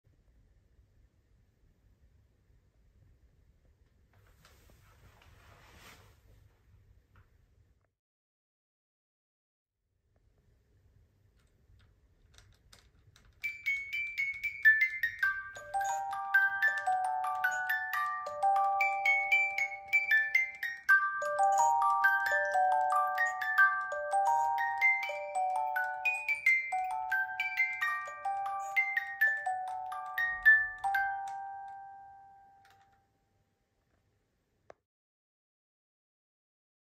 "i agree", 2023
text object with music organ,
acrylic glass, epoxy, mirror